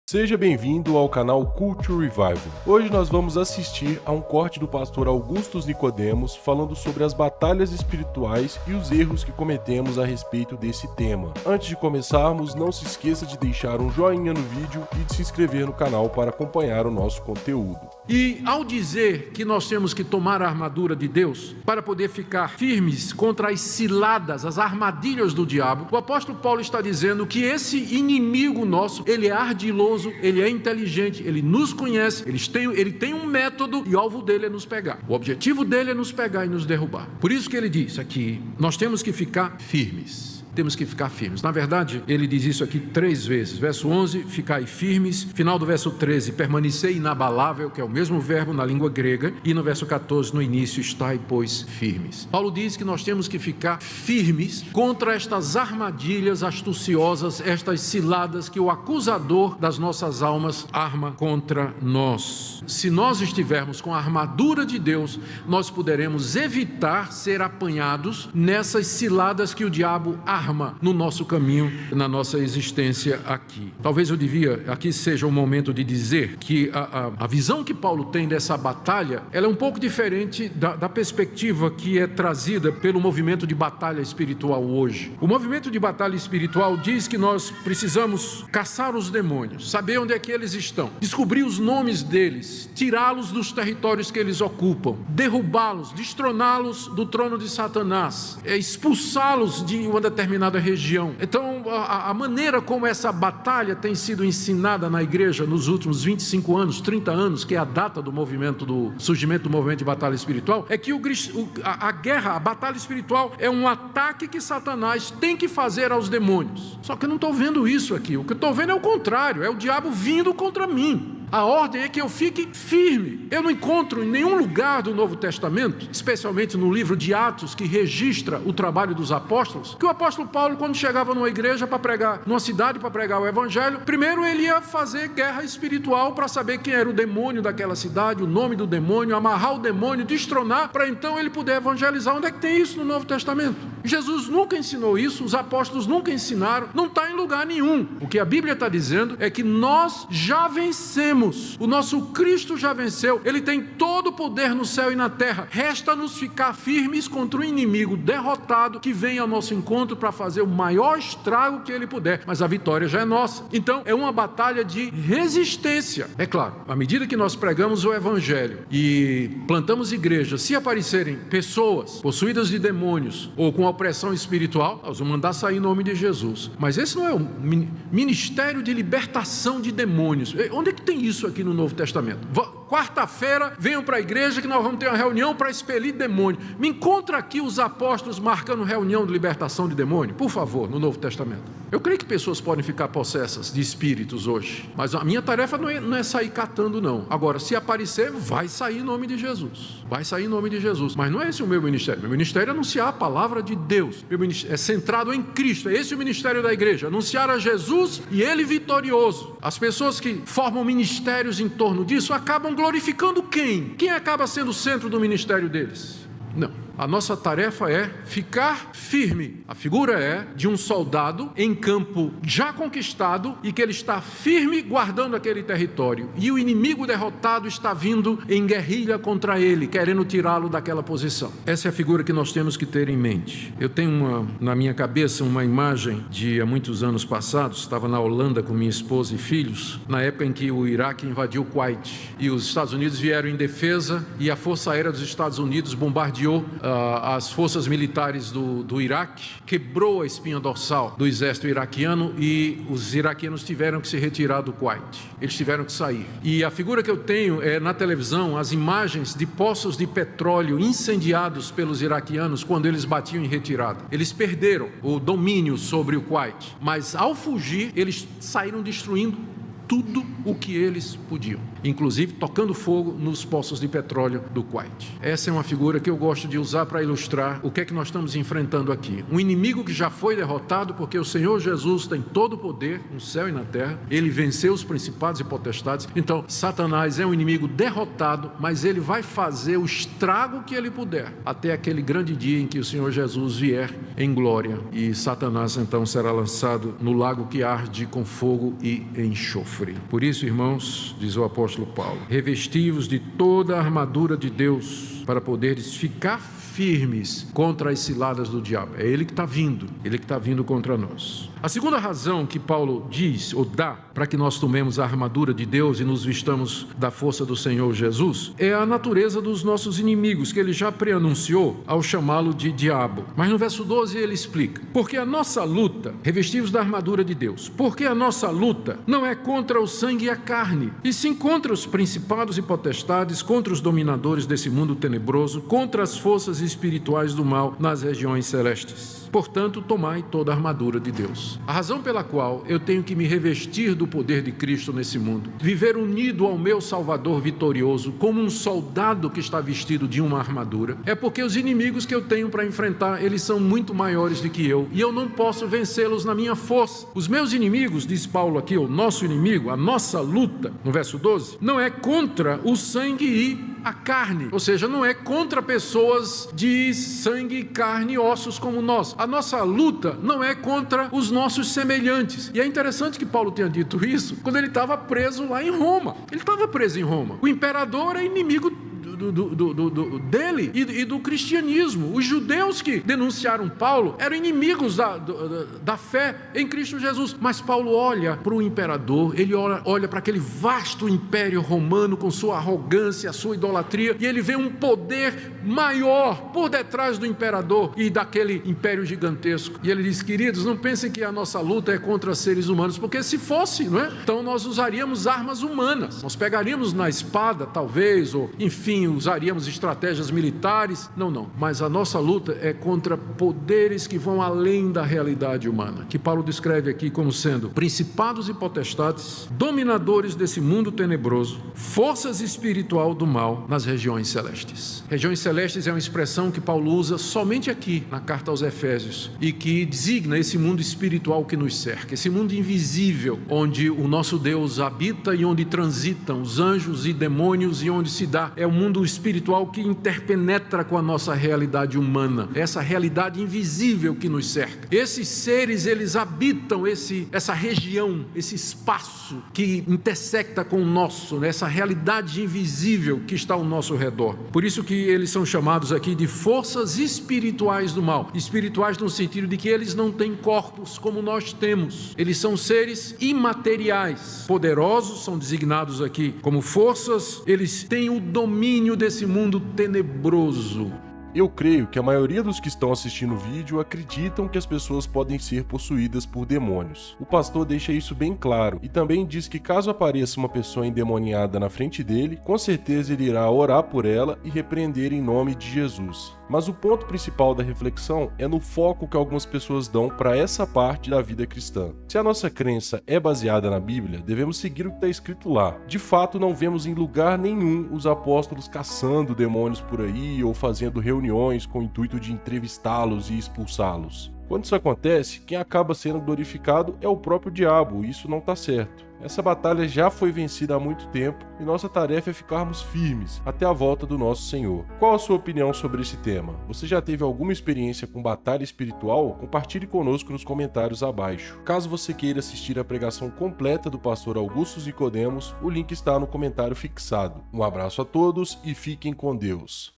COMO FUNCIONA A BATALHA ESPIRITUAL? | Augustus Nicodemus
O Rev. Augustus Nicodemus fala como funcionam as batalhas espirituais e como os cristãos devem se portar diante delas. Nessa pregação, ele demonstra onde alguns crentes têm errado diante do mundo espiritual.